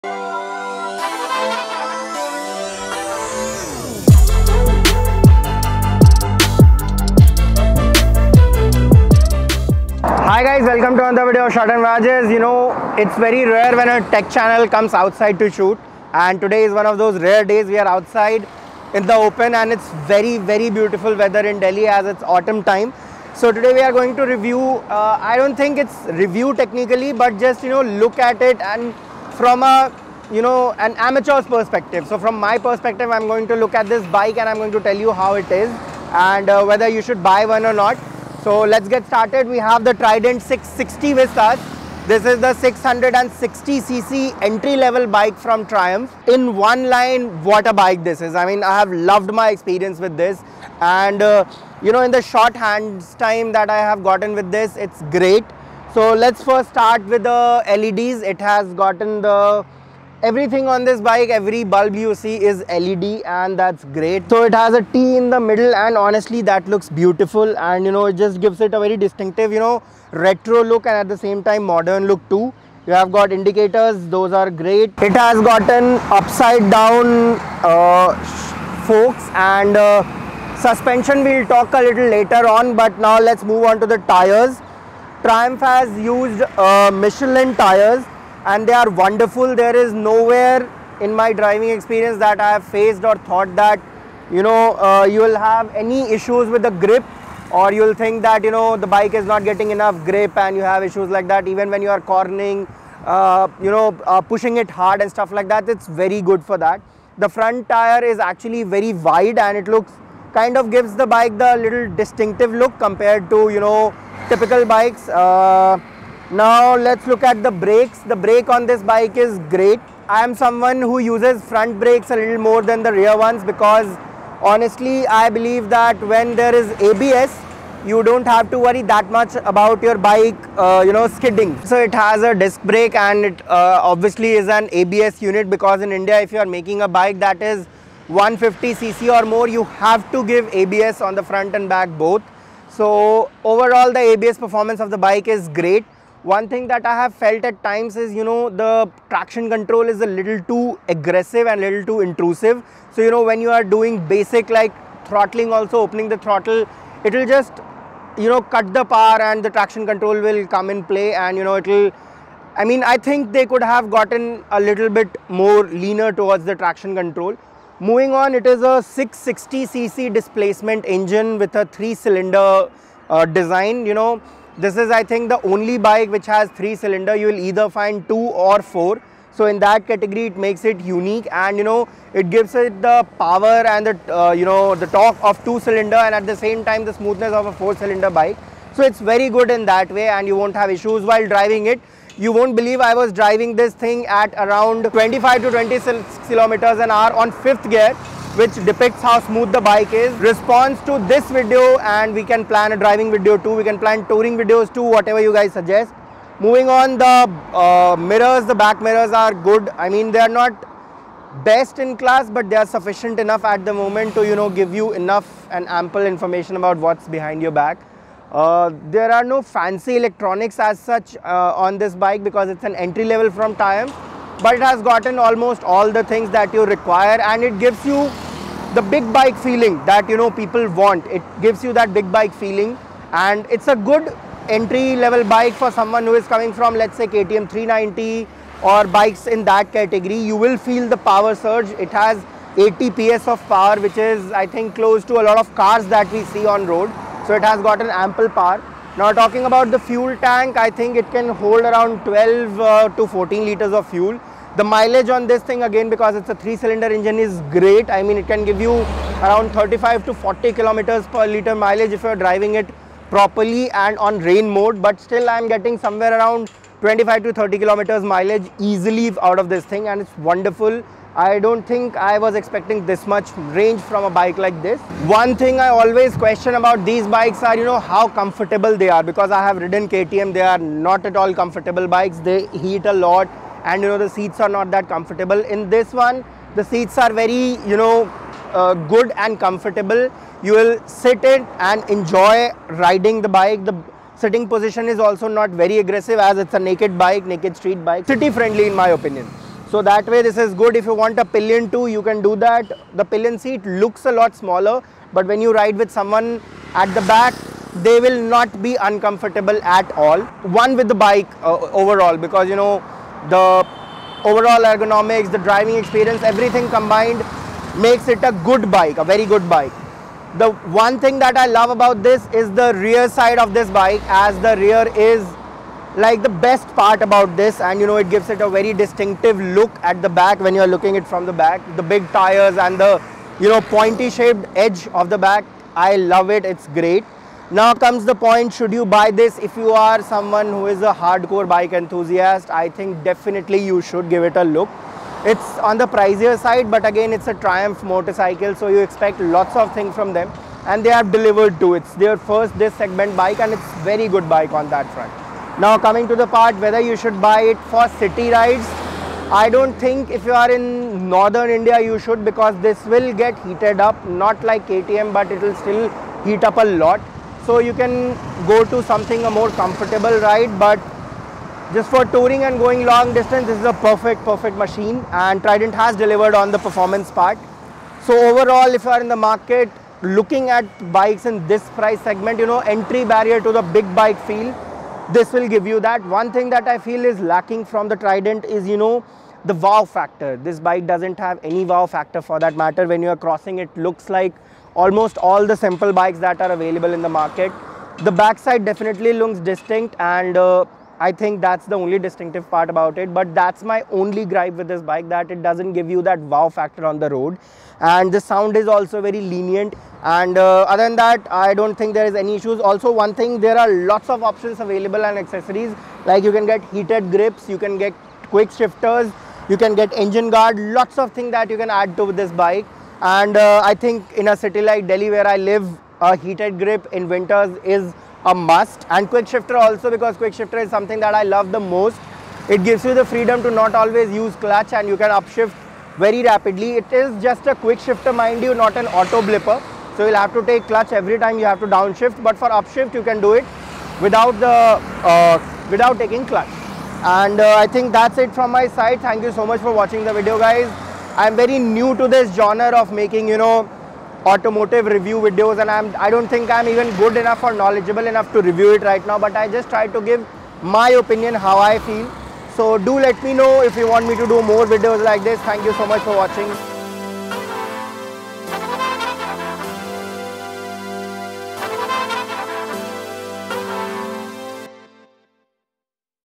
Triumph Trident 600: The Friendly Roadster
The triumph trident 660 is a good entry point for someone who is looking to get into big bikes. It offers great power with great confidence.